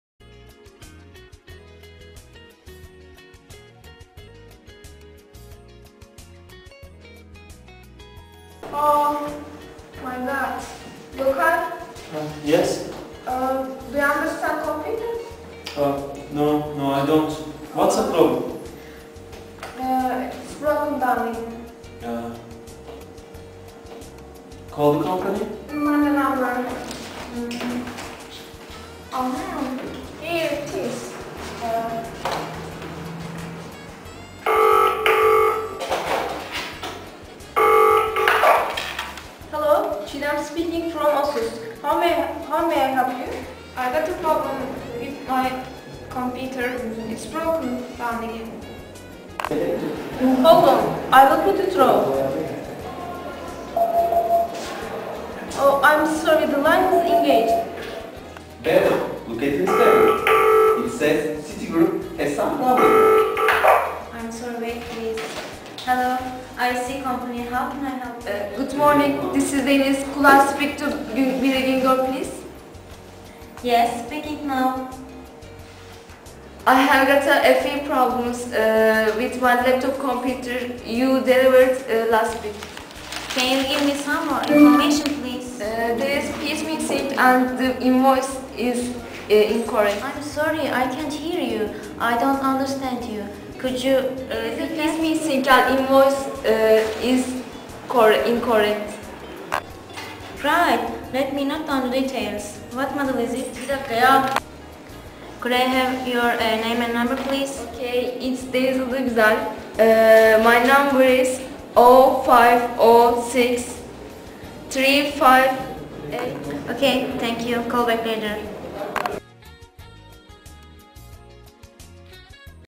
ceren kaplan has got a problem with her computer
how to deal with office problems on th ephone